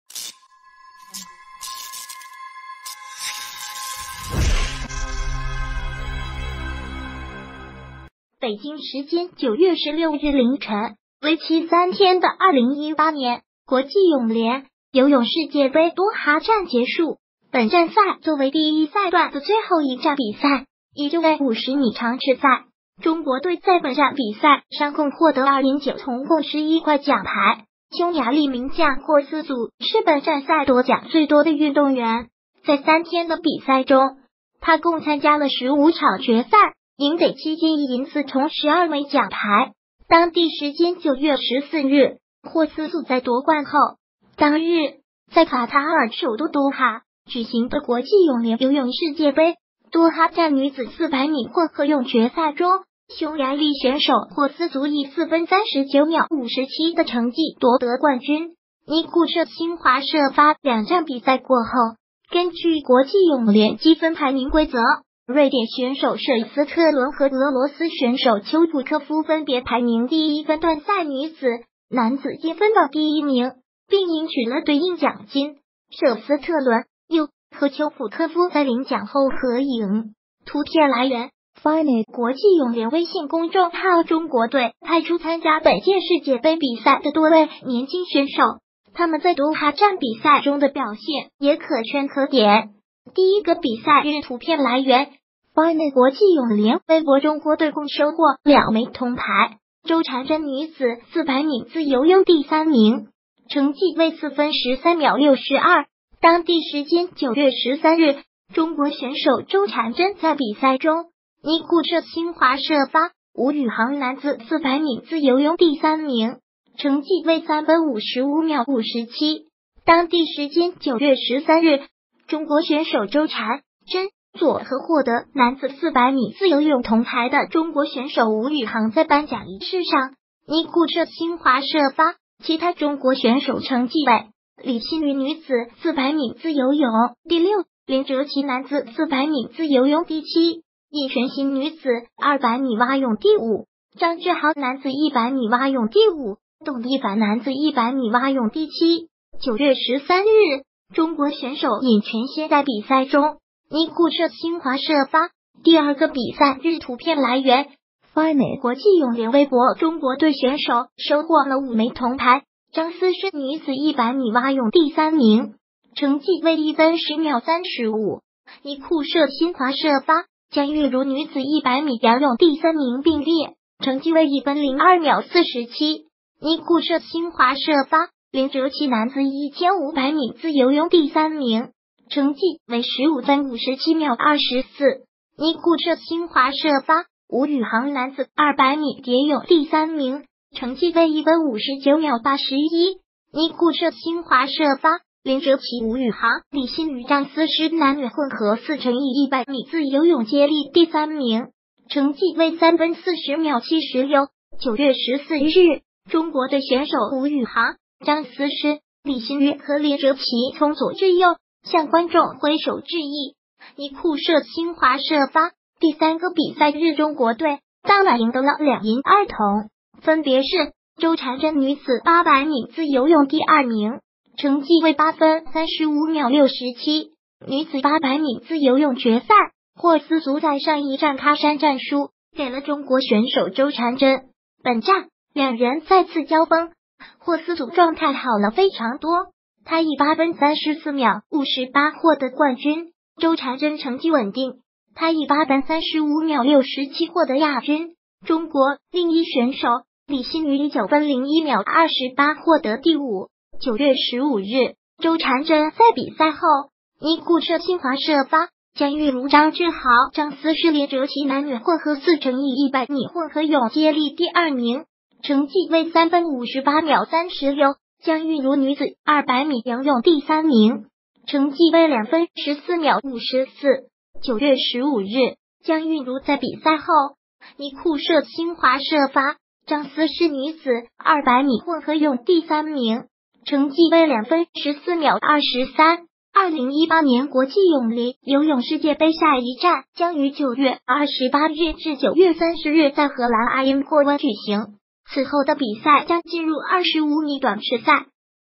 游泳世界杯多哈站：中国队年轻选手收获2银9铜
北京时间9月16日凌晨，为期3天的2018年国际泳联游泳世界杯多哈站结束，本站赛作为第一赛段的最后一站比赛，依旧为50米长池赛。中国队在本站比赛上共获得2银9铜共11块奖牌。匈牙利名将霍斯祖是本站赛夺奖最多的运动员，在3天的比赛中，她共参加了15场决赛，赢得7金1银4铜12枚奖牌。当地时间9月14日，霍斯祖在夺冠后。当日，在卡塔尔首都多哈举行的国际泳联游泳世界杯多哈站女子400米混合泳决赛中，匈牙利选手霍斯祖以4分39秒57的成绩夺得冠军。尼库摄（新华社发） 两站比赛过后，根据国际泳联积分排名规则，瑞典选手舍斯特伦和俄罗斯选手丘普科夫分别排名第一分段赛女子、男子积分榜第一名，并赢取了对应奖金。舍斯特伦（右）和丘普科夫在领奖后合影。 图片来源：FINA国际泳联微信公众号中国队派出参加本届世界杯比赛的多位年轻选手，他们在多哈站比赛中的表现也可圈可点。第一个比赛日图片来源：Fina国际泳联微博中国队共收获两枚铜牌：周婵臻女子400米自由泳第三名，成绩为4分13秒62。当地时间9月13日，中国选手周婵臻在比赛中。 尼库摄（新华社发）吴宇航男子400米自由泳第三名，成绩为3分55秒57。当地时间